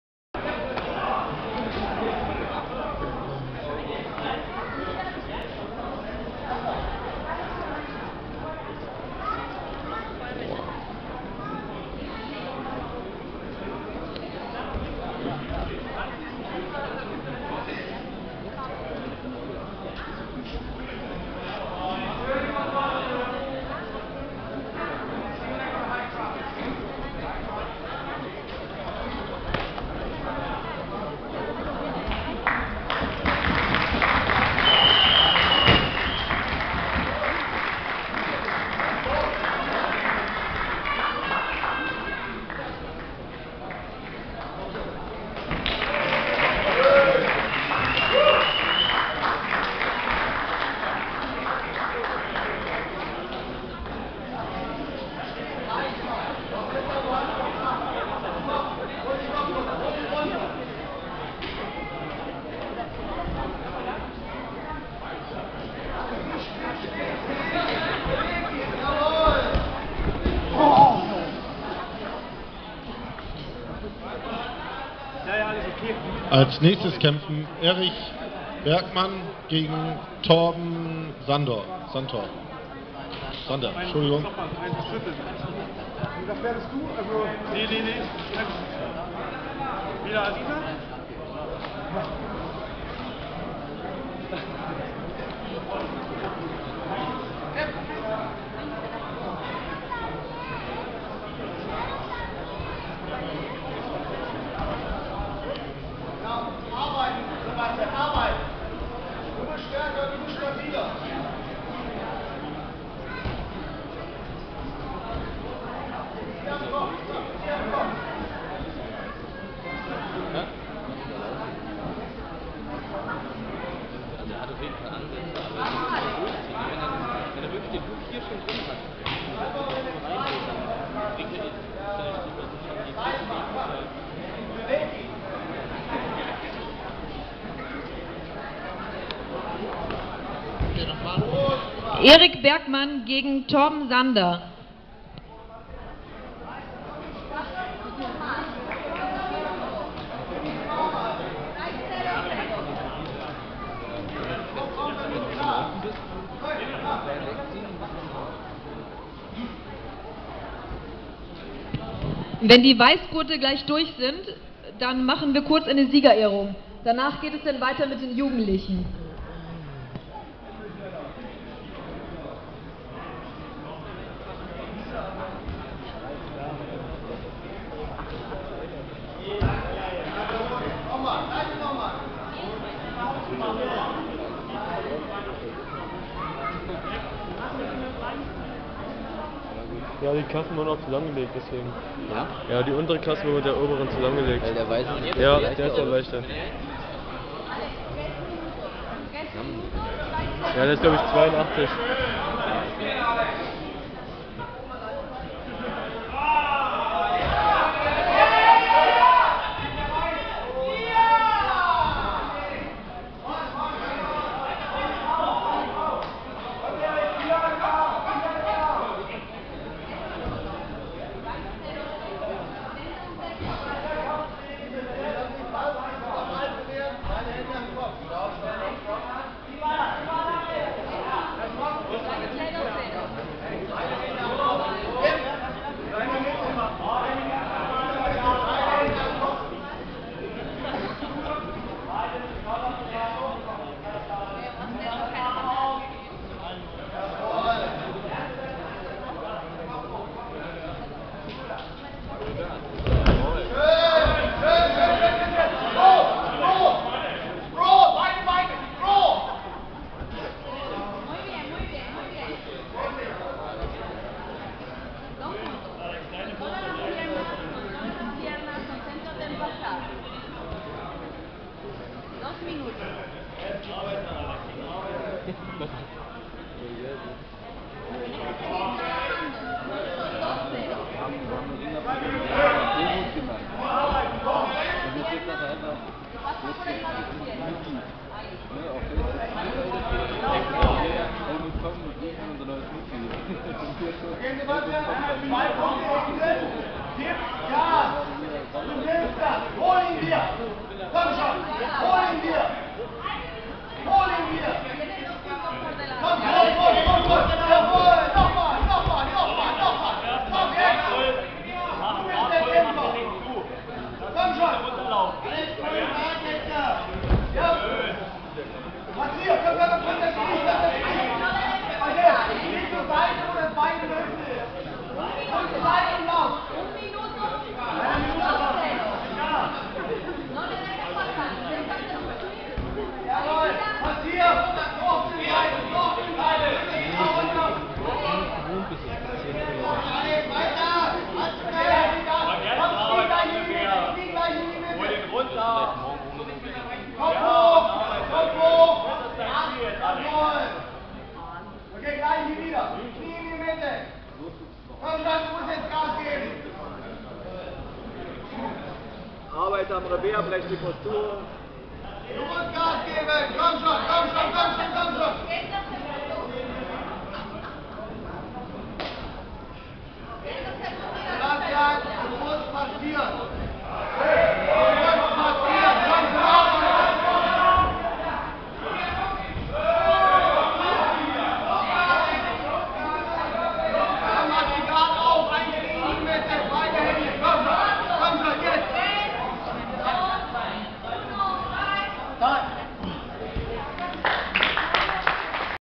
4. Hamburg Open Alexander "Kaiser" Mikus vs Sebastian Risch
Semi Final BJJ Hamburg Open 2013 Kaiser vs Sebastian Risch (German MMA fighter) bringing it on in a stand up fight